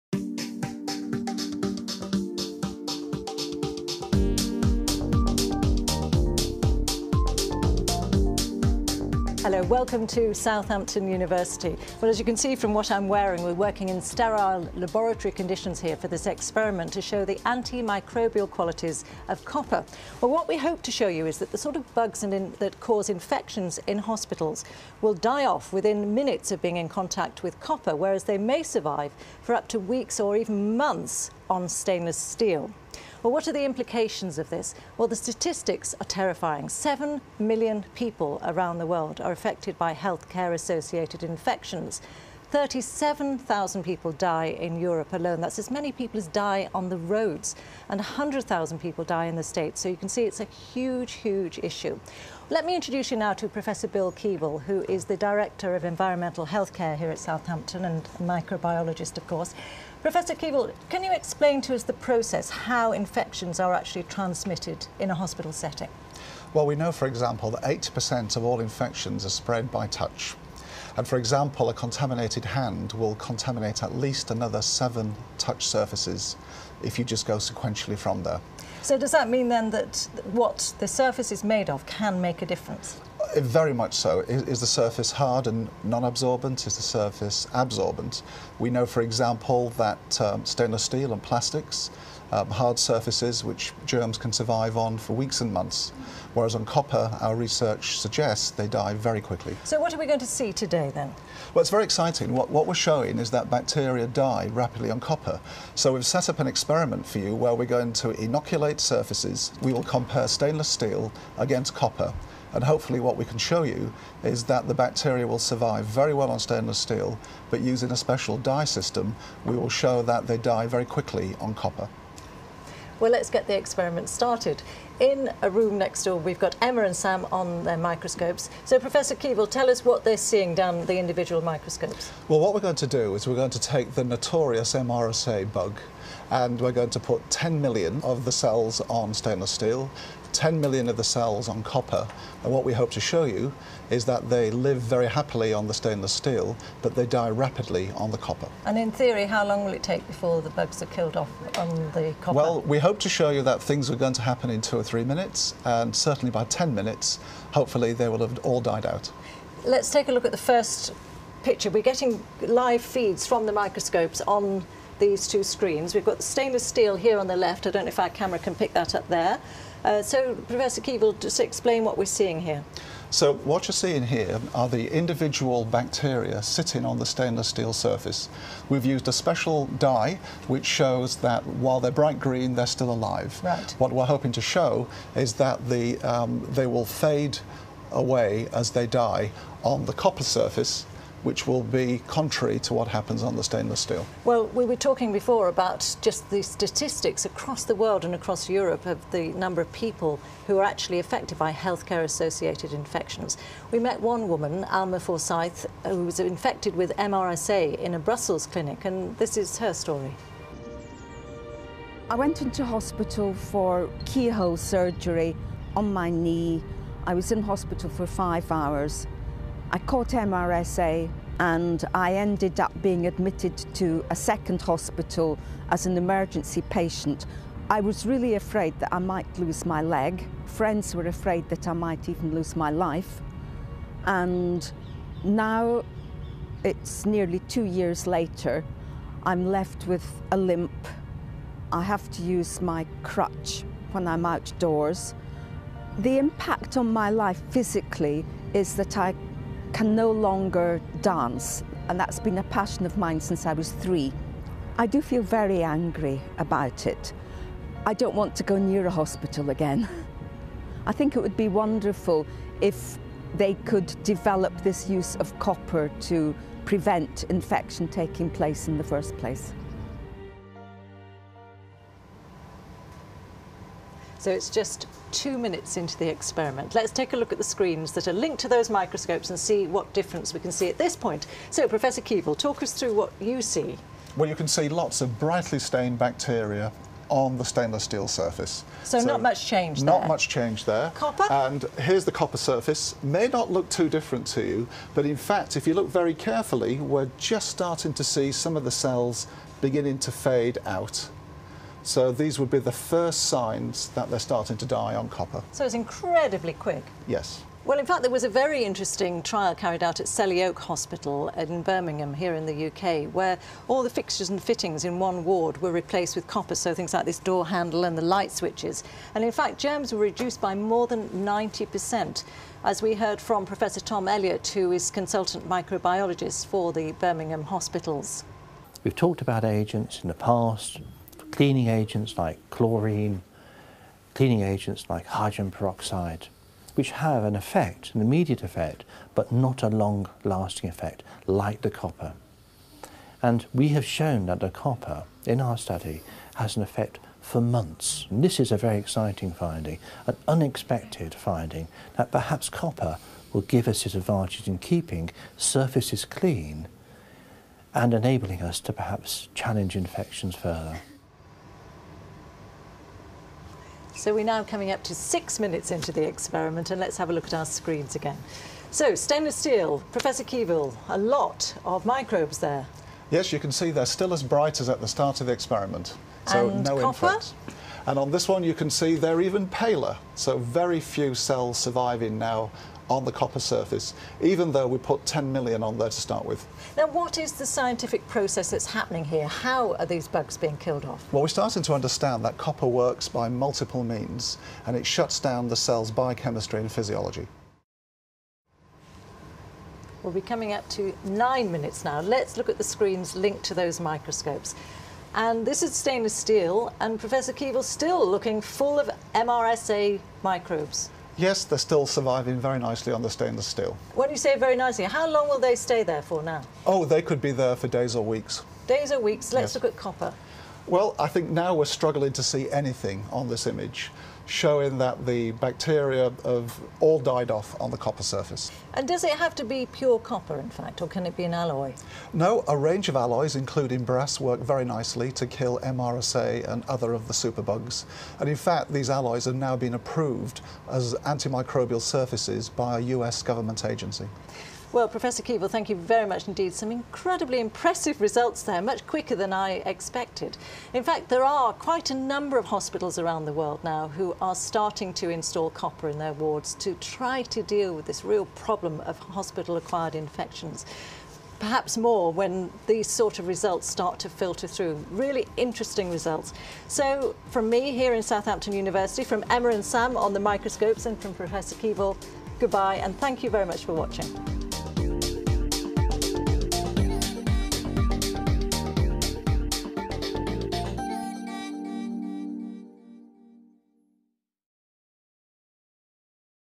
10 - Demonstração ao vivo do cobre antimicrobiano
Apresentação ao vivo de experiência do uso de cobre antimicrobiano em superfícies de contato em ambiente hospitalar.